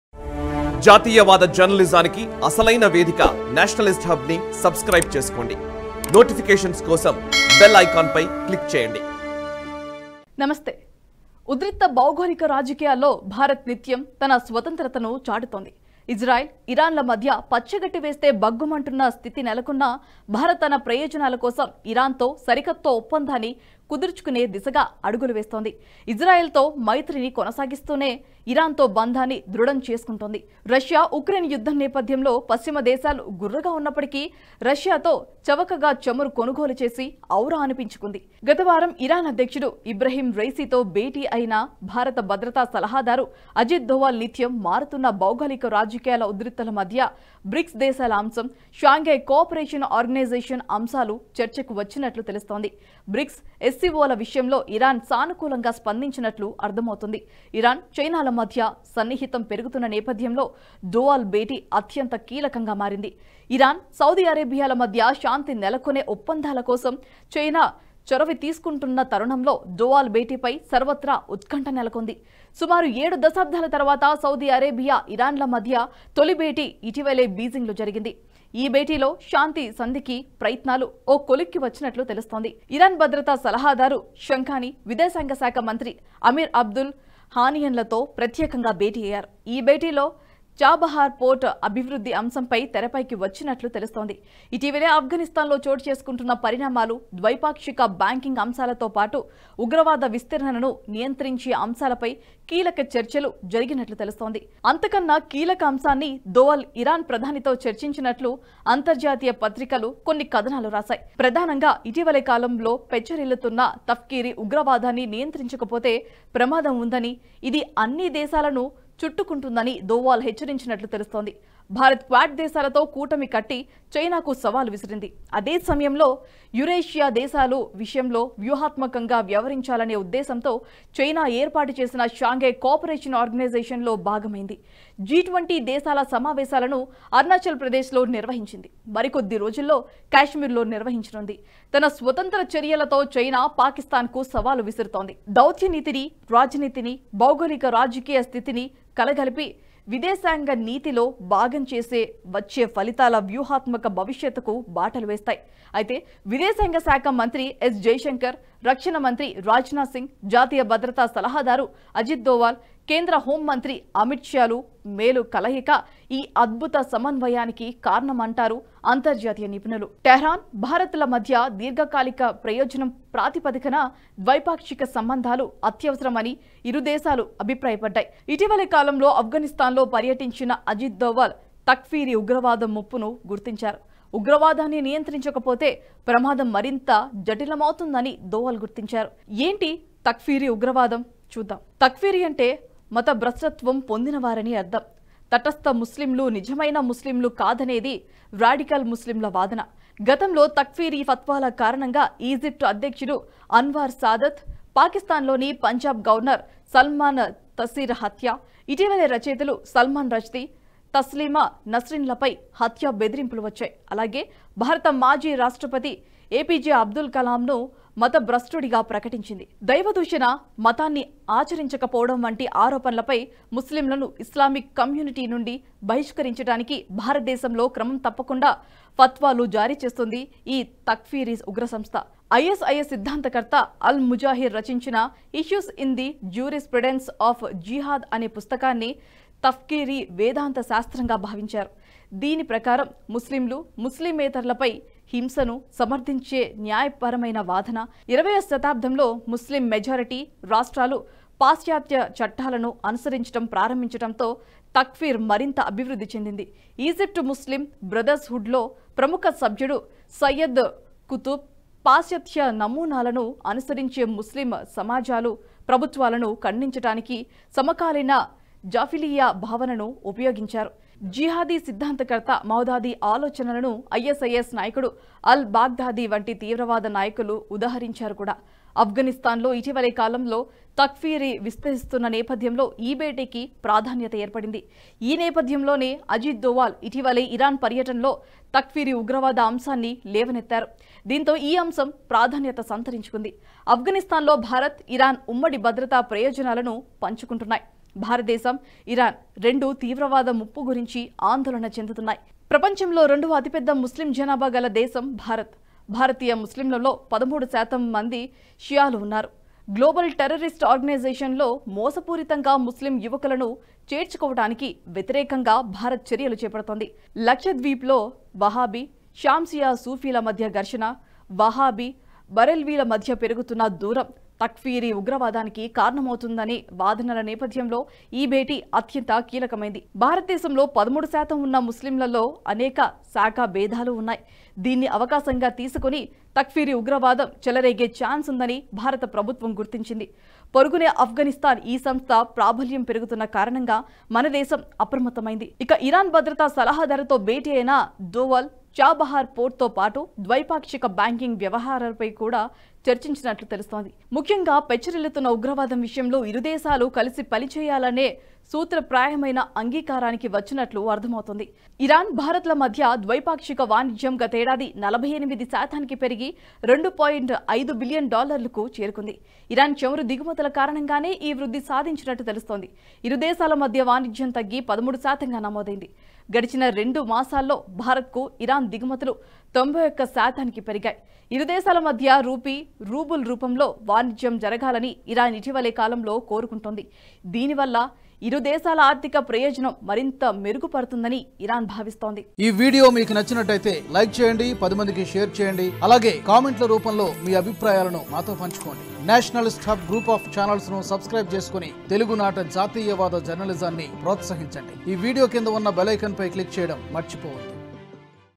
NSA Ajit Doval Visits Iran :India-Iran Ties Moving to a New Level | China | PM Modi |Nationalist Hub
Our Company account details :
IFSC Code: HDFC0000512